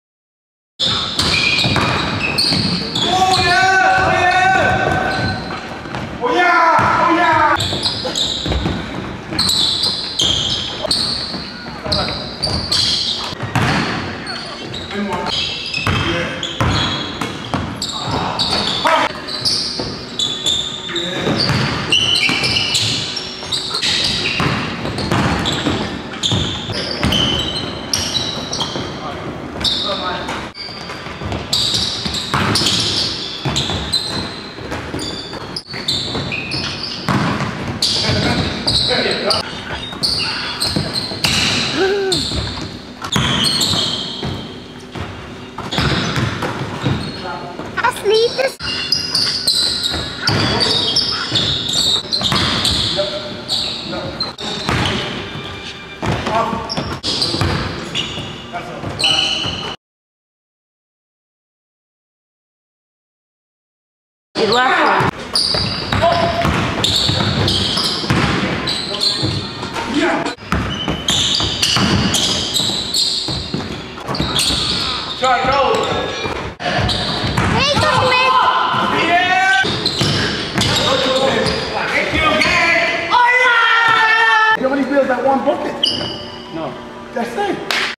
These older dudes thought they had those those boys cooked. See what happened
Epic basketball display of skills and trash talk. Just when the OGs thought they had the game in the bag, the YGs had different ideas